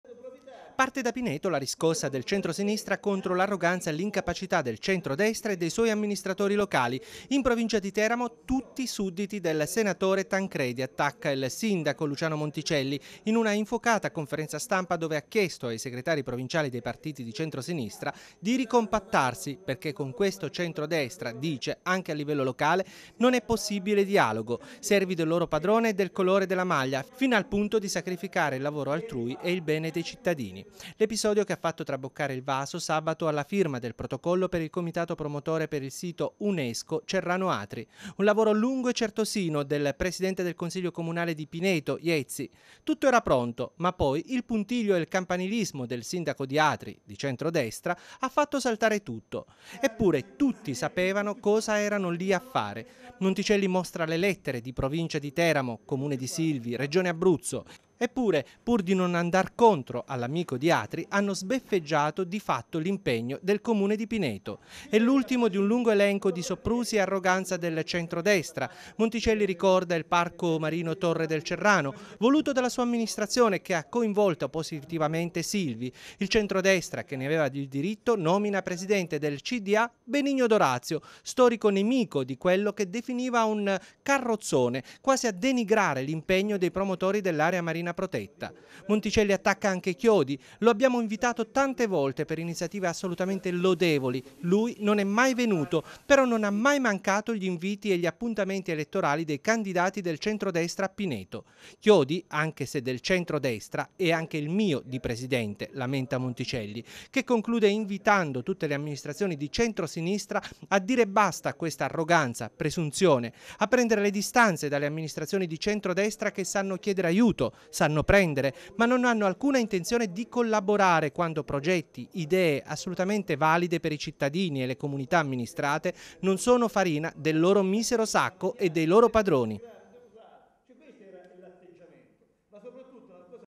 Parte da Pineto la riscossa del centro sinistra
Il territorio non ne può più dell'arroganza, presunzione e incapacità degli amministratori locali del centro destra. È il duro attacco lanciato oggi dal sindaco di Pineto Luciano Monticelli